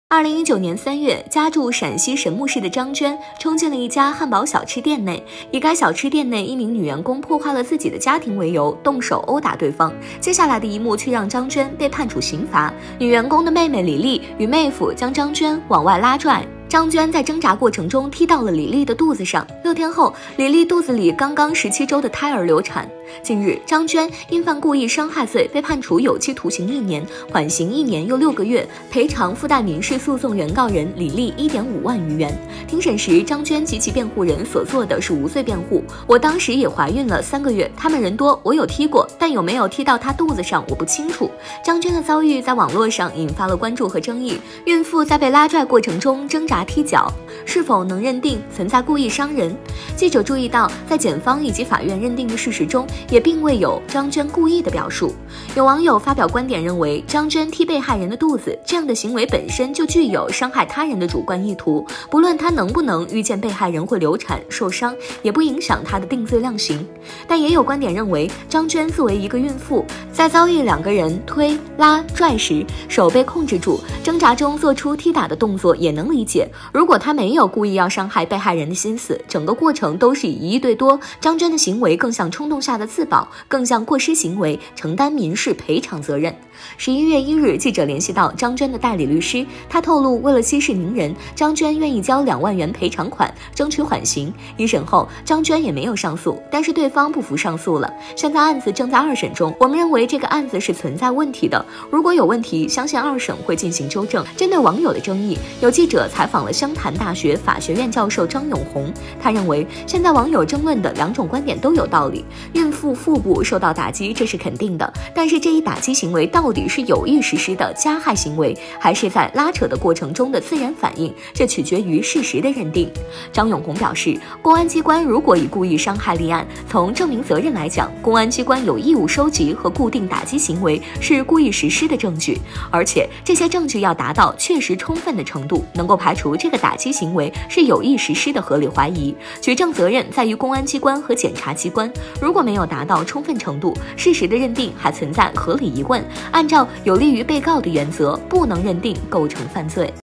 丈夫出轨，女子殴打其情人致劝架孕妇流产，是否故意伤人引争议
2019年3月，家住陕西神木市的张娟（化名）冲进了一家汉堡小吃店内，以该小吃店一名女员工破坏了自己的家庭为由，动手殴打对方。